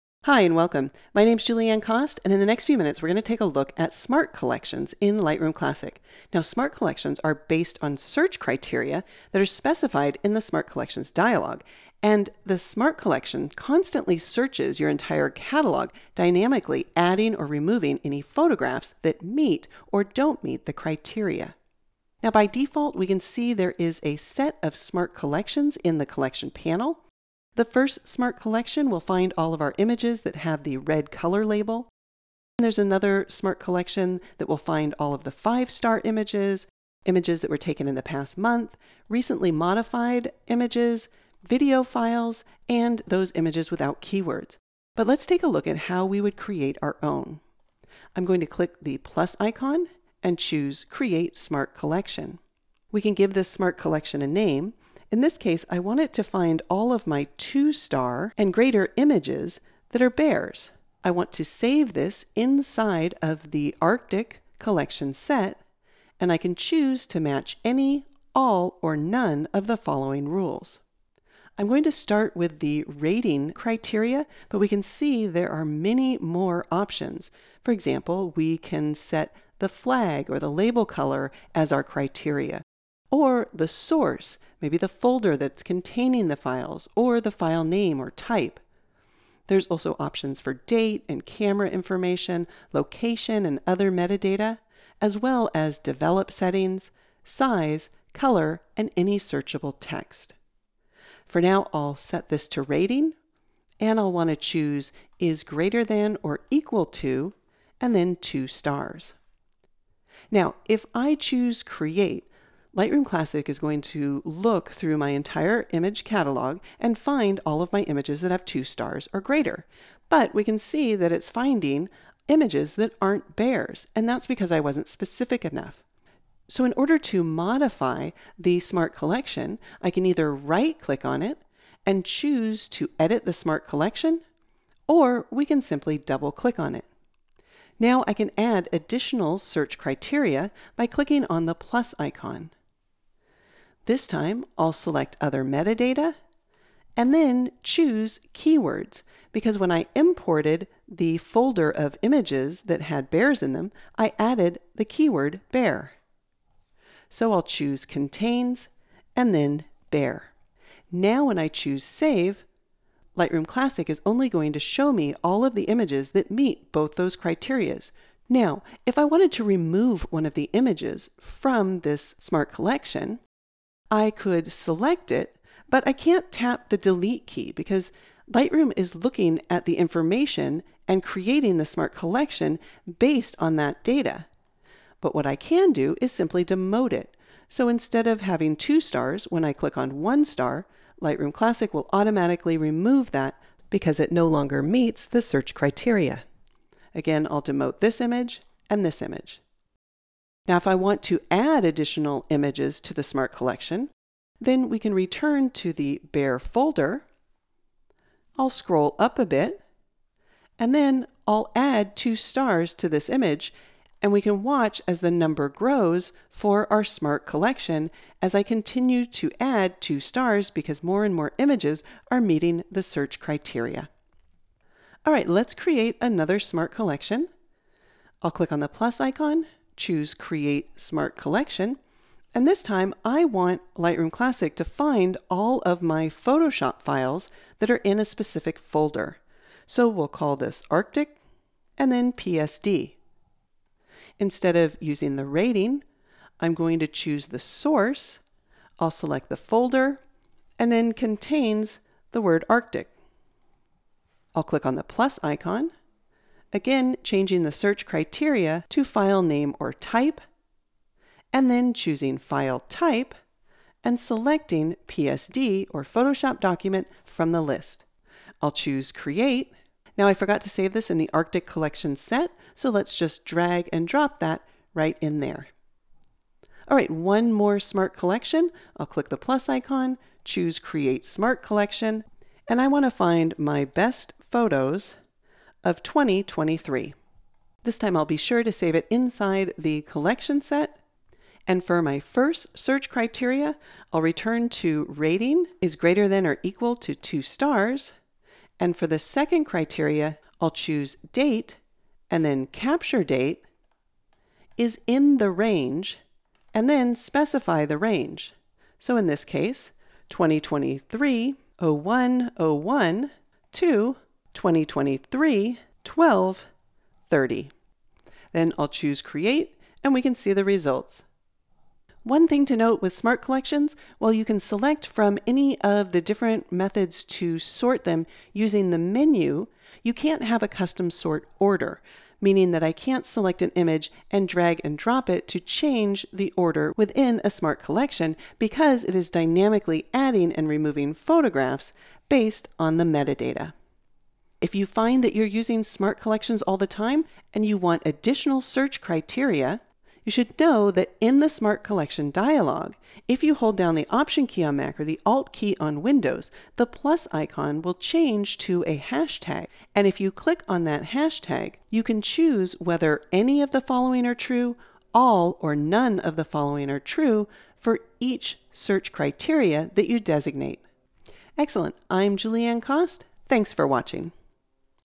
Working with Smart Collections in Lightroom Classic
Discover how to use search criteria and Smart Collections to quickly find your photos in Lightroom Classic.